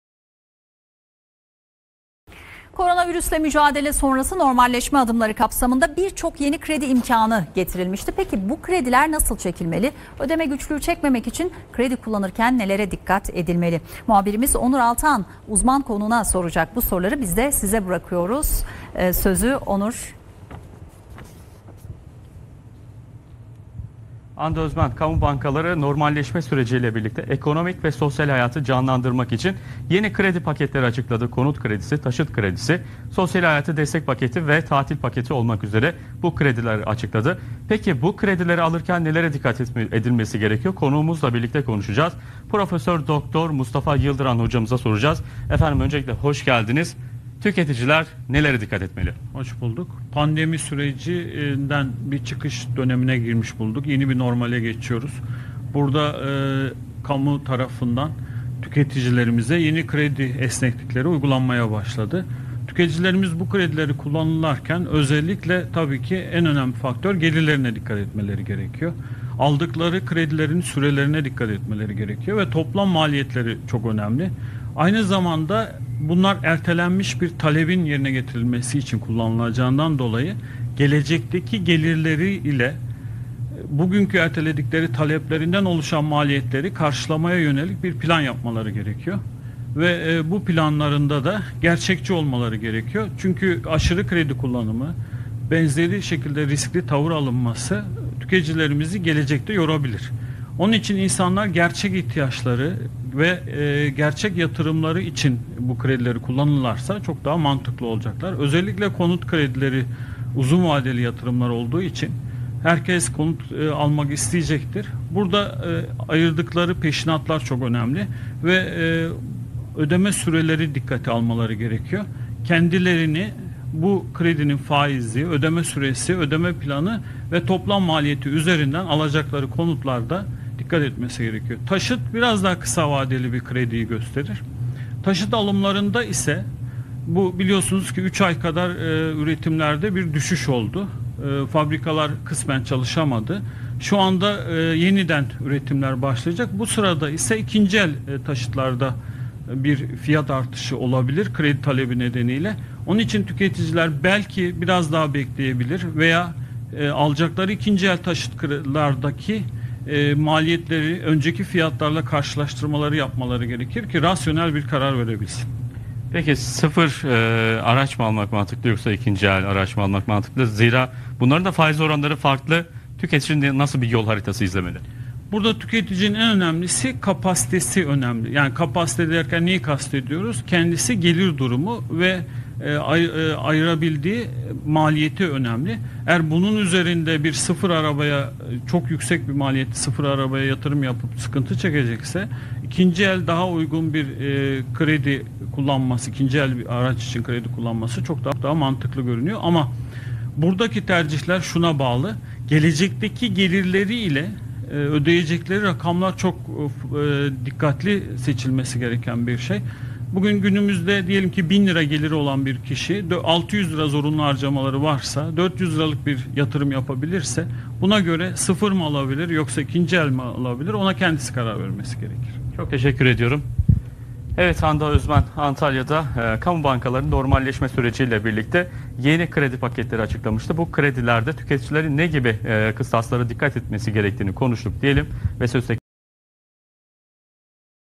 Kredi Kullanırken Nelere Dikkat Edilmeli?
Akdeniz Üniversitesi İktisadi ve İdari Bilimler Fakültesi Maliye Bölümü Mali İktisat Anabilim Dalı Öğretim Üyesi Prof. Dr. Mustafa Yıldıran, TRT Haber canlı yayınında kredi çekecek vatandaşa uyarılarda bulundu. Yıldıran, kredi çekerken kısa vadeli ve küçük oranlarda kredi kullanılması gerektiğini söyledi.